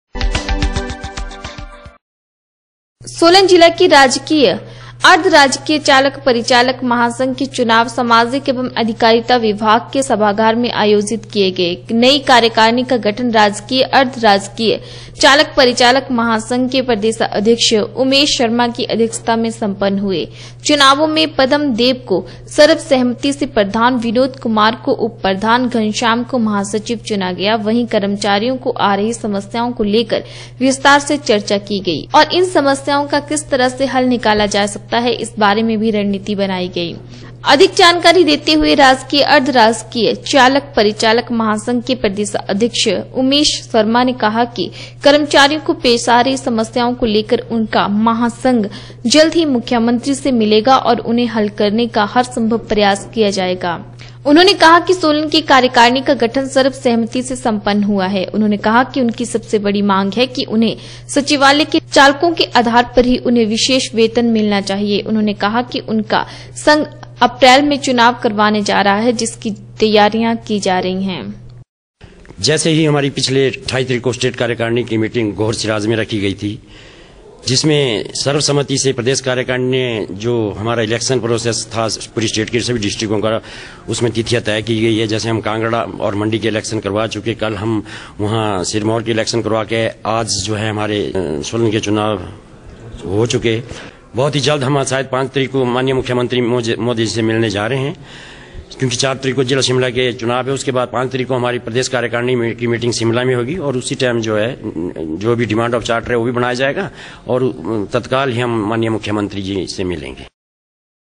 जिला सोलन की राजकीय अर्धराजकीय चालक परिचालक महासंघ के चुनाव हुए सम्पन्न
जिला सोलन की राजकीय अर्धराजकीय चालक परिचालक महासंघ के चुनाव सामाजिक एवं अधिकारिता विभाग के सभागार में  आयोजित किये गए .नई कार्यकारणी का गठन राजकीय अर्धराजकीय  चालक परिचालक महासंघ के प्रदेशाध्यक्ष  उमेश शर्मा की अध्यक्षता में सम्पन्न  हुए | चुनावों में पदम् देव को सर्व सम्मति से प्रधान , विनोद कुमार को उपप्रधान ,घनश्याम को महासचिव   चुना गया |